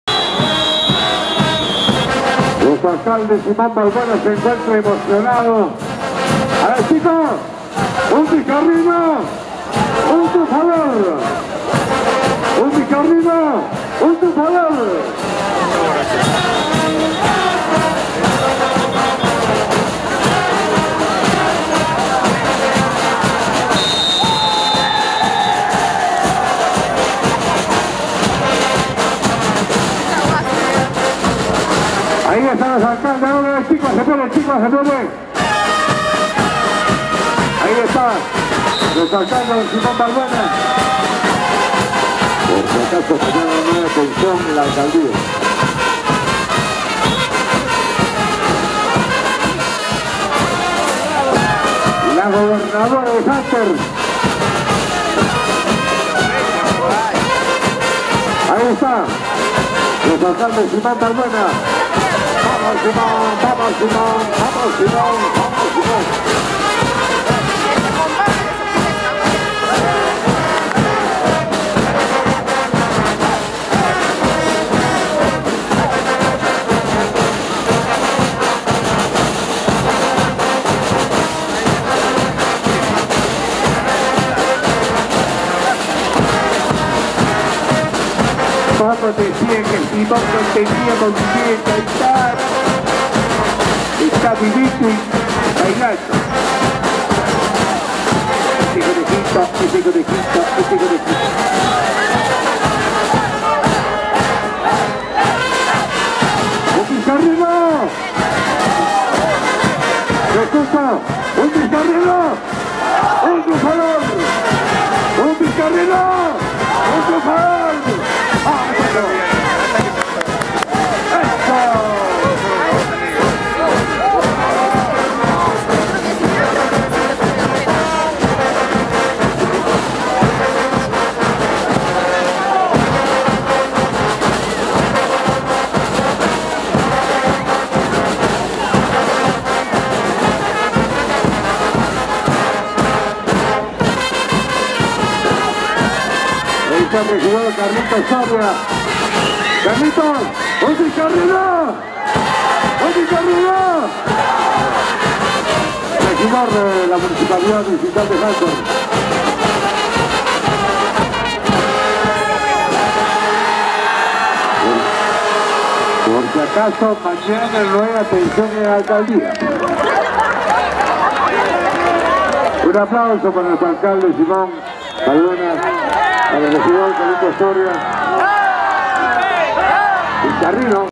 Autoridades bailando Carnaval de Chachas con la I.E. Juan Pablo Vizcardo y Guzmán
Alcalde del Distrito de Hunter don Simón Balbuena Marroquín
Gobernadora de Hunter Sra. Elena Verónica Lipa Mamani
Regidor Sr. Carlos Alfredo Huarca Castro
Sr. Jimmy R Ojeda Arnica
durante el Gran Corso de la Amistad por las "Bodas de Plata" de Creación Política del distrito Jacobo Hunter, realizado el día Domingo 07 de Junio del 2015 en la Av. Las Américas.